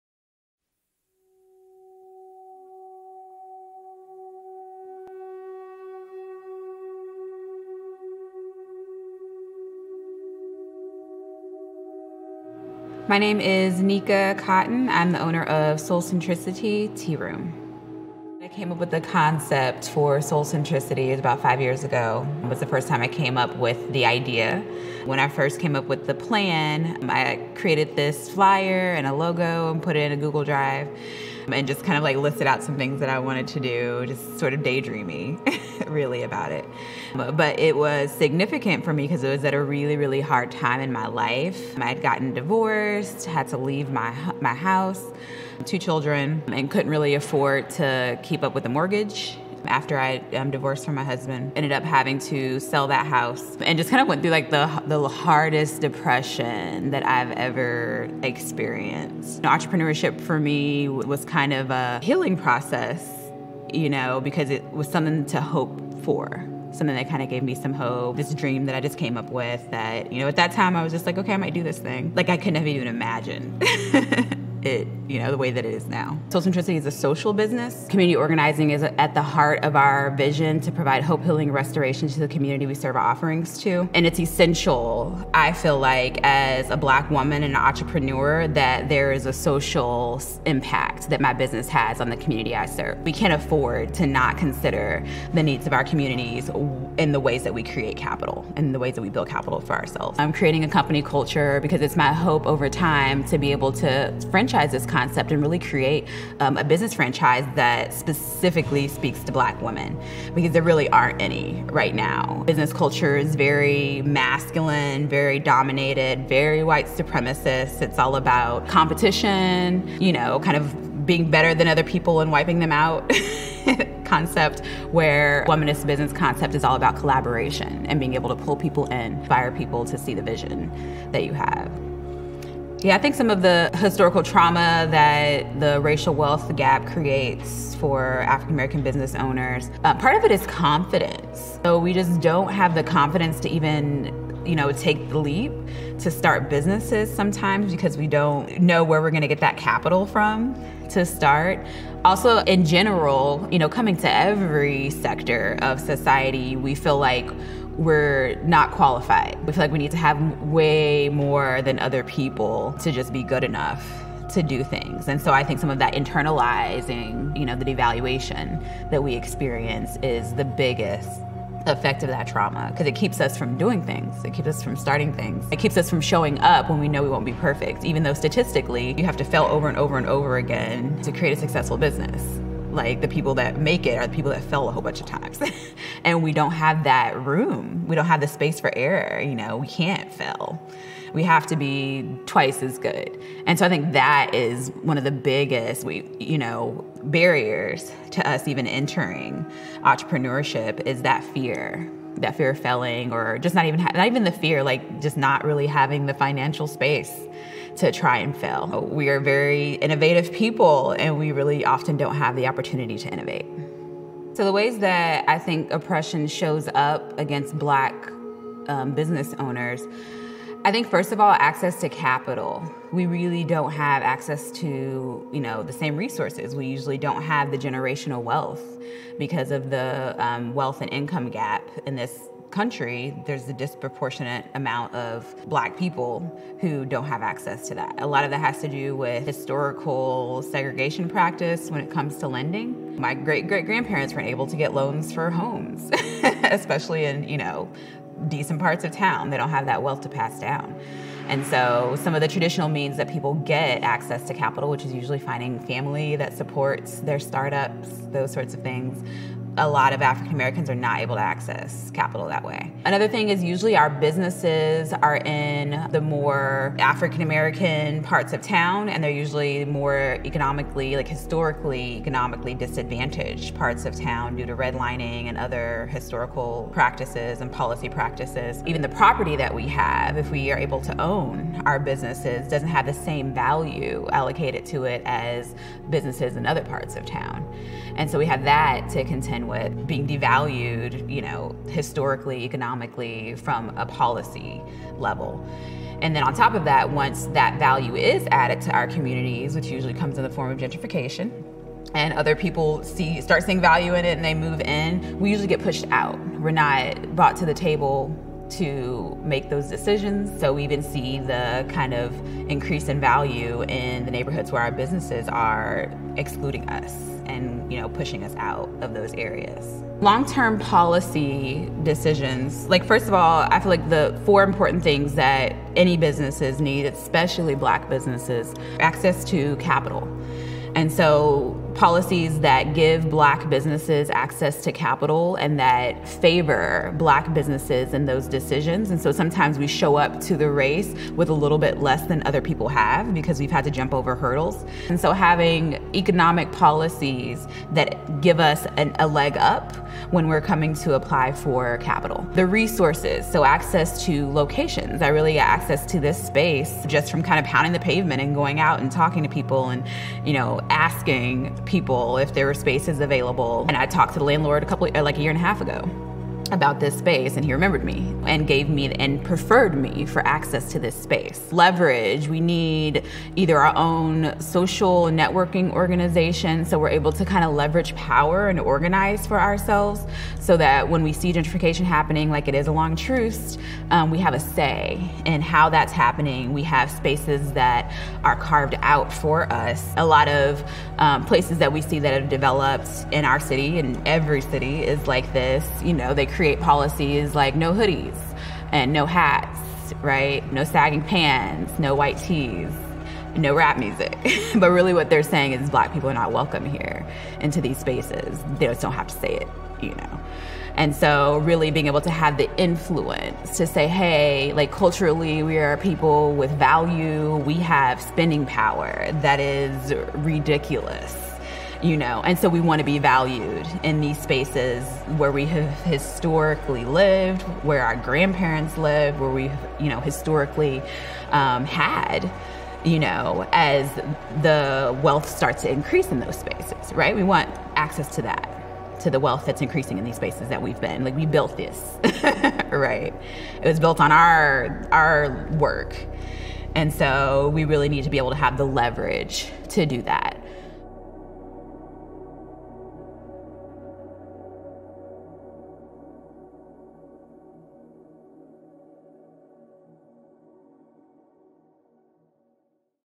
Creating Change Through Black Women Entrepreneurship | Nika Cotton | Alive KC Initiative
Nika Cotton is the owner of Soulcentricitea Tea Room on Troost in Kansas City, MO. As a single mother, she decided to start a socially-conscious business in 2020 that is grounded in womanist practices and activism. Her mission is to provide hope, healing, and restoration to the community. She is actively empowering Black business owners and bringing their voices to the table to resist gentrification and leverage capital. To learn how to get involved with the work, visit alivekc.org